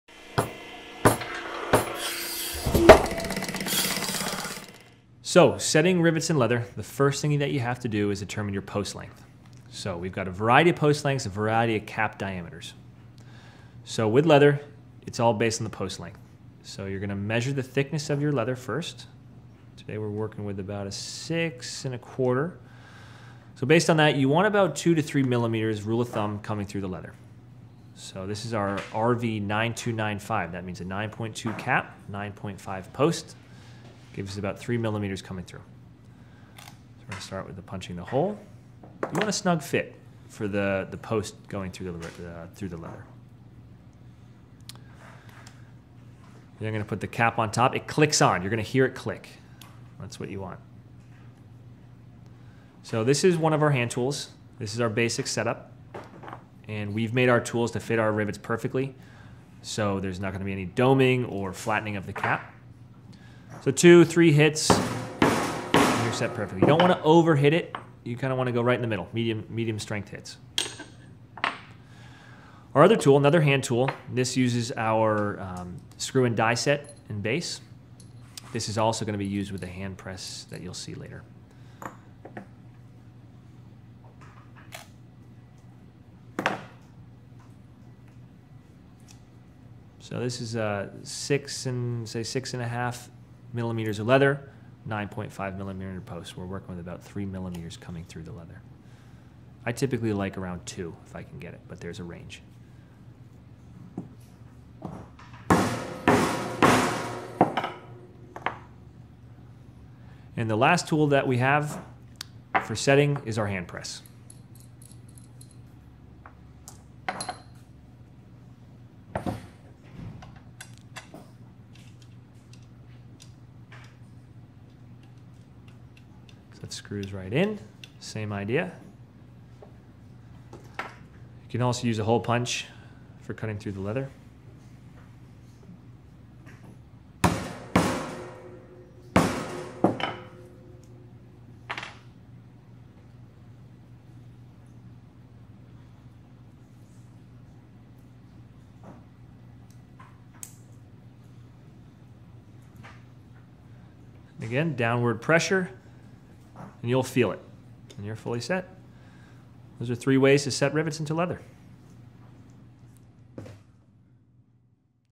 How To Set Rivets in Leather
How To Set Double Cap Rivets in Leather & How to Choose the Right Rivet

We will show you the many ways on 1) how to set rivets in leather, fabric, canvas, and more, as well as 2) how to determine the right rivet size based on your material thickness.

We'll also go over how to use the rivet hand tool setters, a hand press and the hole punches that go along with it! As you grow in your leathercraft, we're here to grow with you with upgraded tools that can increase your speed, consistency, and fingers!.

As always, with any further questions feel free to give us a call or email!